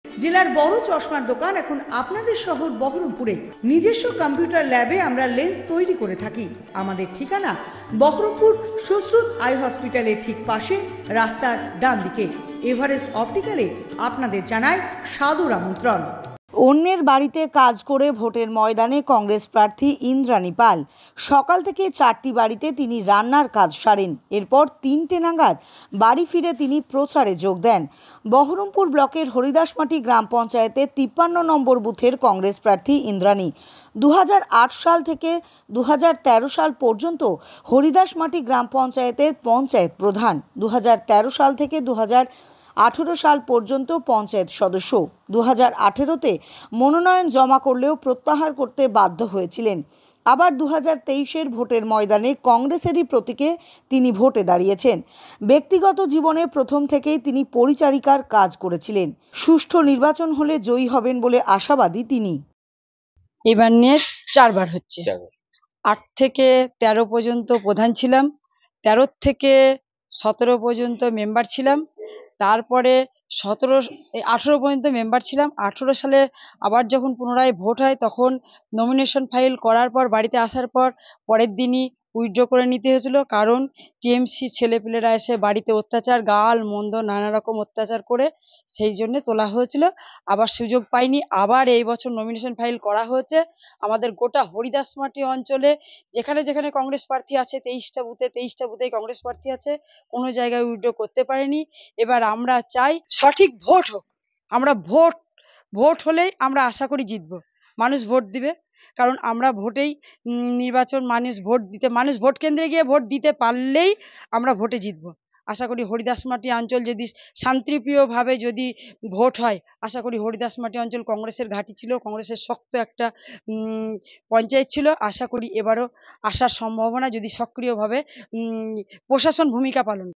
Congress: ব্যাতিক্রমি প্রাক্তন পঞ্চায়েত প্রধান, অন্যার বাড়িতে রান্না করে ভোটের ময়দানে ইন্দ্রানী
২০০৮ সাল থেকে ২০১৩ সাল পর্যন্ত হরি দাস বাটি গ্রাম পঞ্চায়েতের পঞ্চায়েত প্রধান। ২০১৩ সাল থেকে ২০১৮ সাল পর্যন্ত পঞ্চায়েত সদস্য। ২০১৮ মনোনয়ন জমা করলেও প্রত্যাহার করতে বাধ্য হয়েছিলেন। আবার ২০২৩ এর ভোটের ময়দানে কংগ্রেসের প্রতীকেই তিনি দাঁড়িয়েছেন।